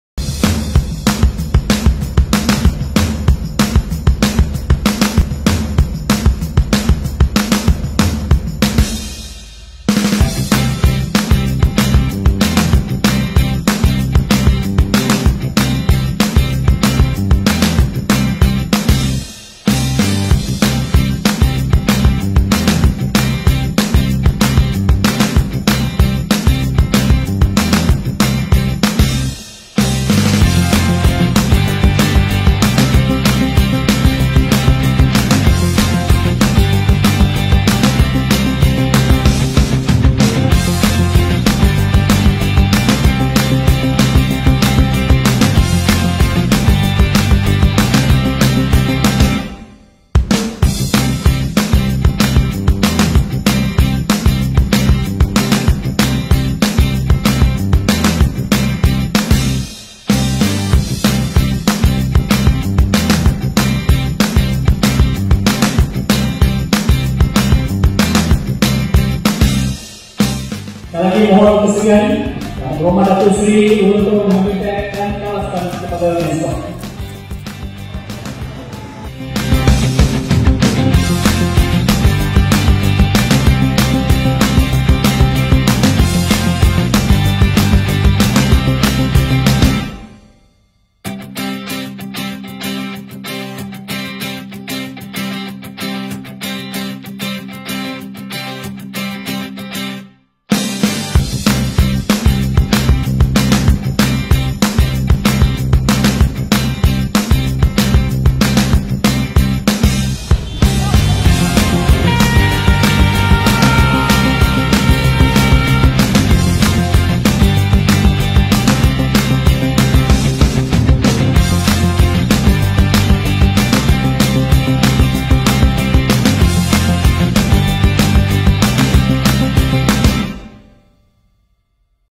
PROGRAM BANTUAN MASUK SEKOLAH AHLI TAWAS 2015
Program Bantuan Masuk Sekolah TAWAS telah bermula secara rasmi pasa 4 Disember 2021 yang lalu bertempat di Dewan Rakyat Batu Caves.
Ibu bapa kepada penerima ahli TAWAS boleh membuat semakan kelayakan di terlebih dahulu dan seterusnya tunggu permakluman daripada Pusat Khidmat DUN kawasan masing masing melalui SMS kepada no telefon yang telah didaftarkan di laman sesawang TAWAS.